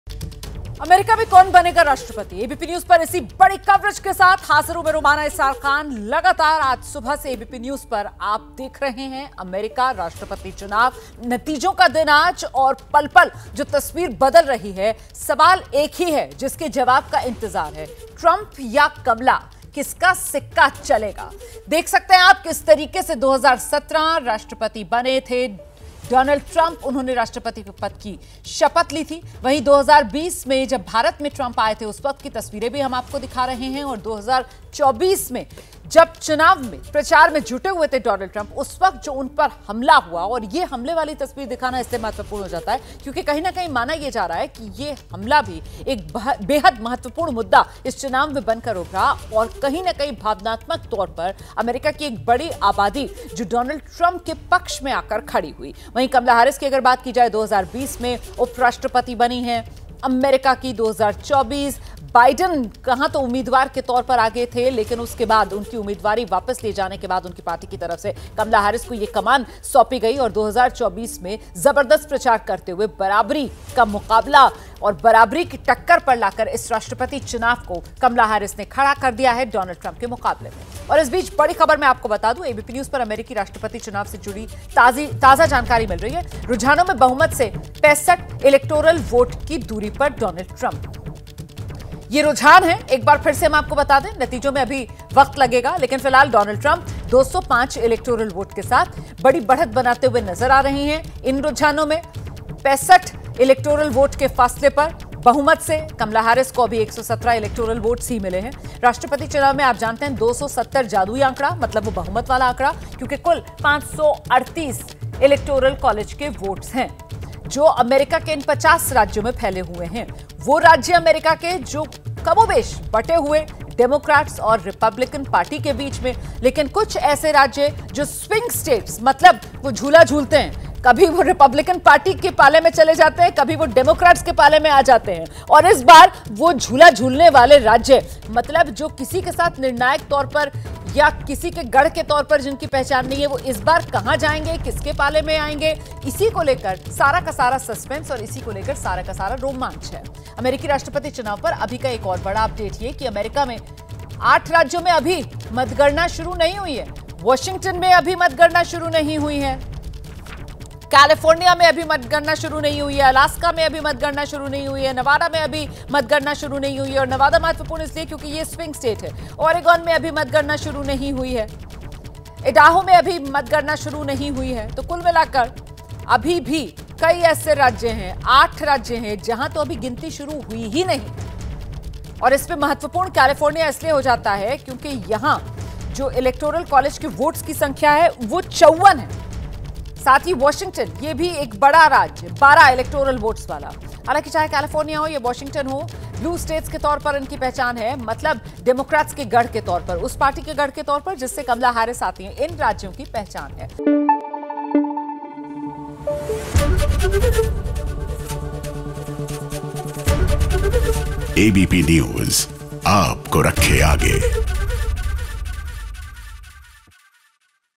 US Presidential Election 2024: रुझानों में बहुमत से 65 इलेक्टोरल वोट दूर Donald Trump | ABP | Harris
US Presidential Election 2024: अमेरिका के सात राज्यों पेंसेल्वेनिया, मिशिगन, विस्कॉन्सिन, जॉर्जिया, नेवादा, एरिजोना और नॉर्थ कैरोलिना को स्विंग स्टेट्स कहा जाता है. ताजा सर्वेक्षणों के मुताबिक जॉर्जिया, नॉर्थ कैरोलिना,पेंसेल्वेनिया और विसकॉन्सिन में डोनाल्ड ट्रंप आगे चल रहे हैं. वहीं, मिशिगन और एरिजोना में डेमोक्रेटिक उम्मीदवार कमला हैरिस आगे चल रही हैं. नेवादा के नतीजे अभी तक सामने नहीं आए हैं.